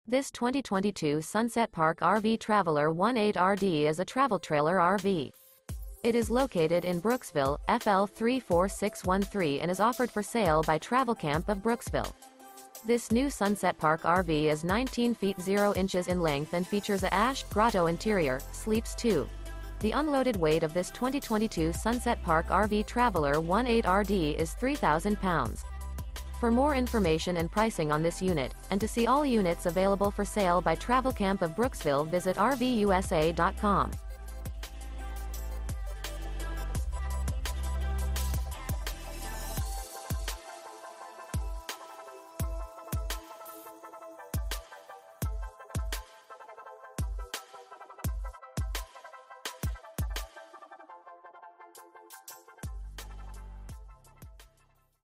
Marvelous 2022 Sunset Park RV Travel Trailer RV For Sale in Brooksville, FL | RVUSA.com
This 2022 Sunset Park RV TRAVELER 18RD is a Travel Trailer RV. It is located in Brooksville, FL 34613 and is offered for sale by Travelcamp of Brooksville. This New Sunset Park RV Is 19' 0" in length and features a ASH/GROTTO interior, sleeps 2.The unloaded weight of this 2022 Sunset Park RV TRAVELER 18RD is 3000 lbs.

Travelcamp of Brooksville is a dealer for brands like: and offers the following rv services: Parts/Accessories, Service. For more information and pricing on this unit, To see all units available for sale by Travelcamp of Brooksville,